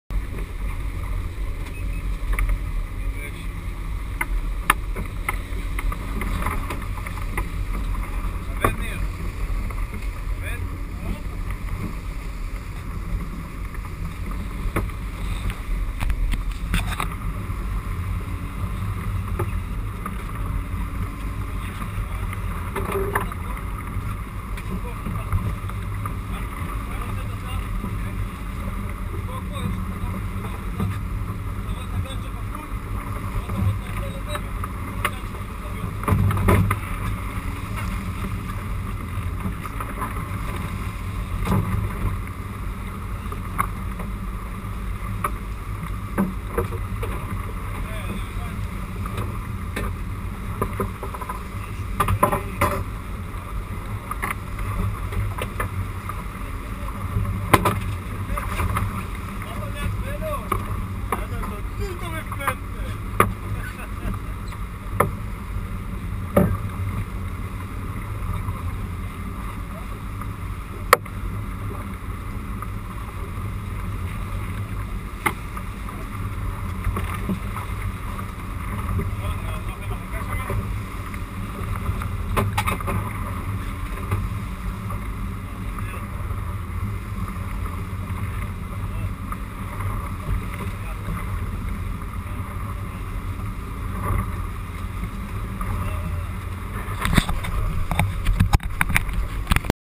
Albacore Tuna Fishing - First Fish Part A - 30 km from Israel Uri Binsted
Albacore Tuna Fishing - First Fish Part A - 30 km from Israel - By Uri Binsted, Tel Bello, Nir Shmueli, Alon Malichi.
Video Shot by my Go Pro HD camera mounted on the boat.